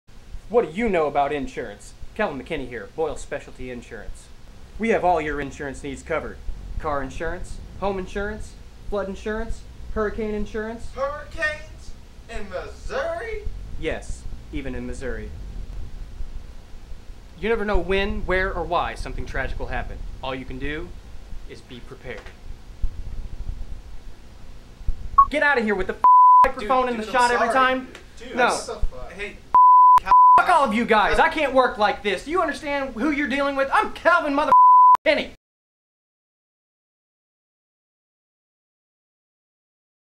Calvin McKinney, Specialty Insurance Agent
Calvin McKinney is passionate about insurance. His drive is to make sure you have the most encompassing and complete insurance policies available to you. He and his partners at Boyle Specialty Insurance Agency work night and day to provide you with up to the minute coverage.

See Calvin again in AUTO(maton) INSURANCE.

Starring David Hafley as Calvin McKinney

Directed by Douglas Wicker

This commercial is intentionally bad.**